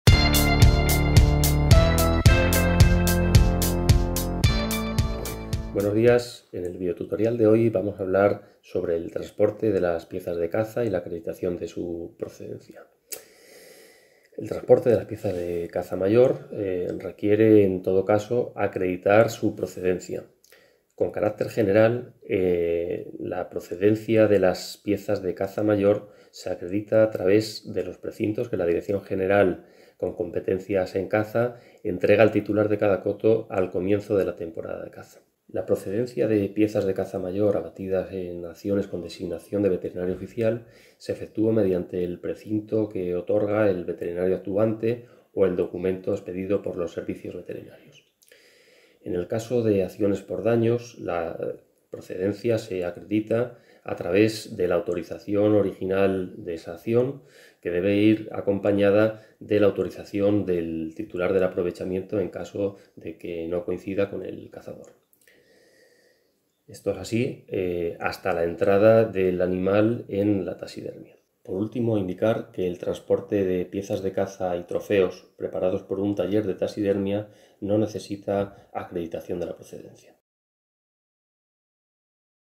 TUTORIAL Procedencia de las piezas de caza
En este nuevo tutorial te contamos cómo se acredita la procedencia de las piezas de caza para su transporte.